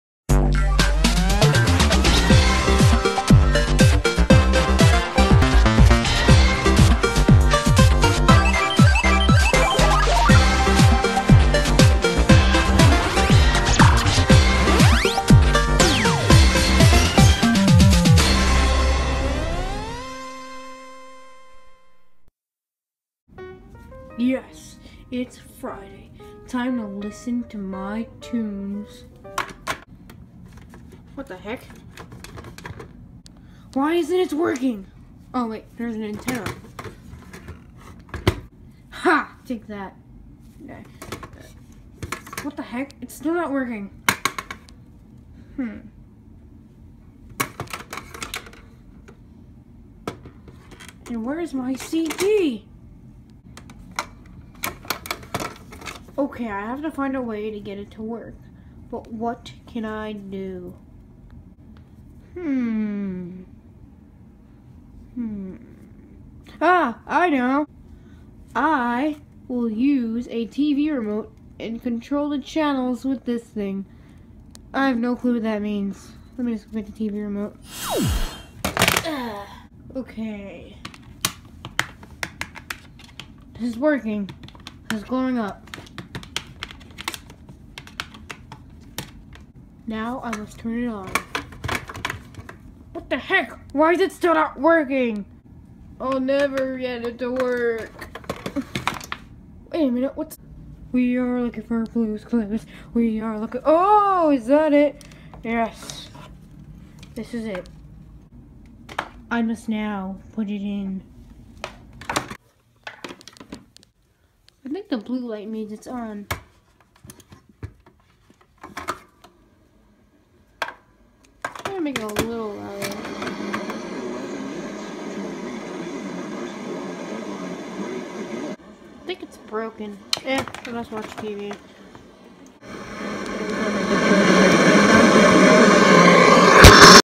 GS Short The Radio
Fender Can't Get His Radio To Work!